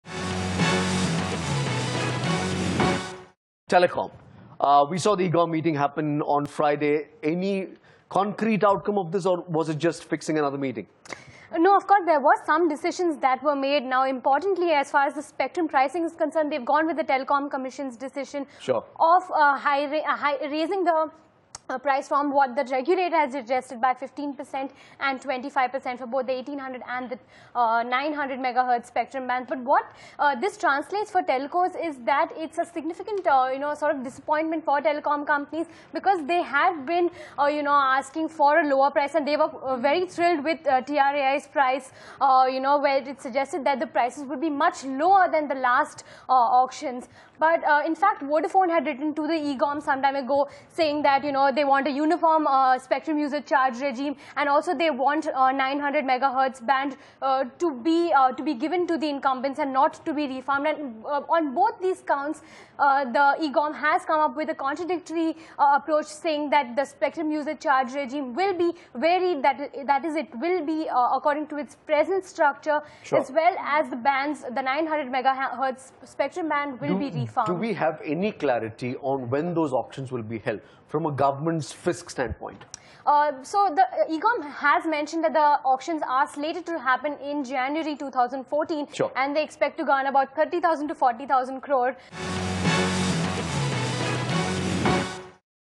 In Business- Spectrum Prices: Minor Relief For Telcos
Friday's EGoM delivered mixed news for the telecom sector. The operators will have to pay higher price for spectrum, but the norms may set the stage for greater consolidation in the sector.

Bloomberg TV India's Priya Sheth shares a detailed report.